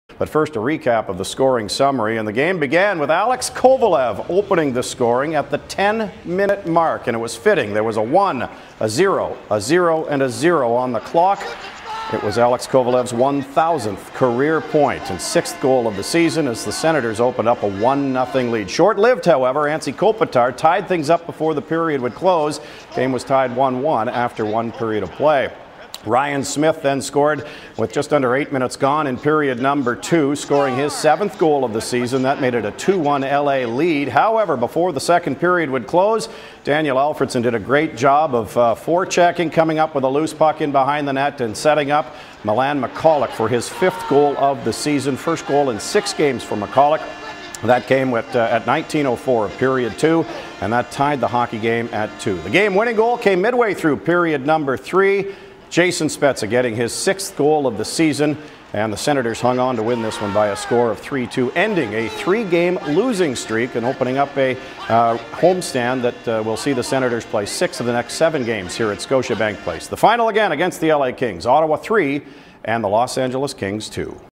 Ottawa Senators vs Los Angeles Kings All Access Highlights, Nov 22, 2010
Gord Brown gives the highlights of the Ottawa Senators 3-2 win over the visiting Los Angeles Kings at Scotiabank Place on Novemeber 22, 2010.

The NHL owns the copyright for all audio and video and the content is uploaded solely for discussion purposes.